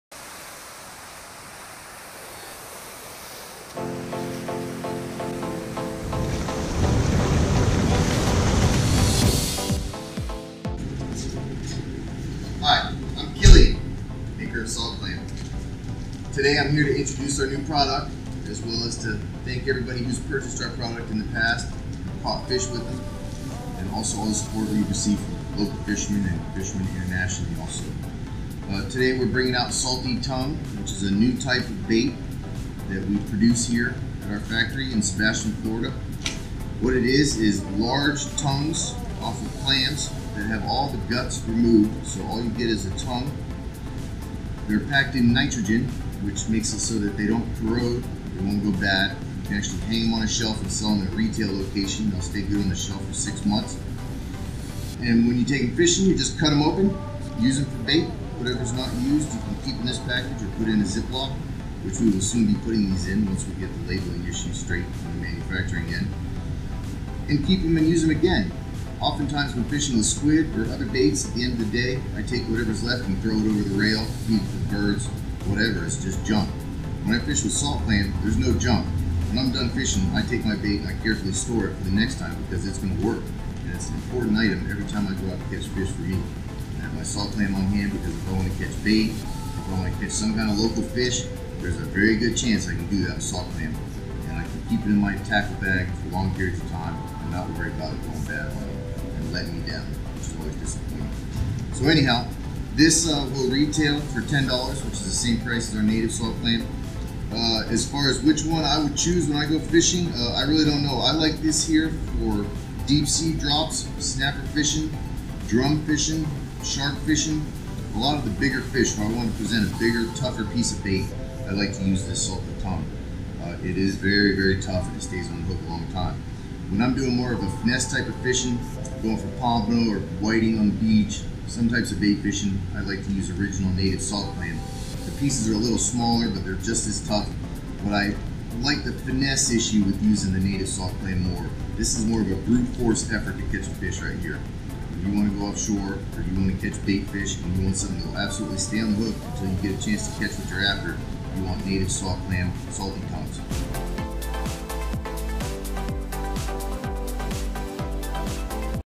Best Fishing Bait Ever: Salty Tongue from Native Salt Clam
Native Salt Clam is excited to announce its latest product, Salty Tongue. This is big bait for big fish, and it stays on the hook so you can catch more fish! Follow Native Salt Clam fishing adventures and fishing tips.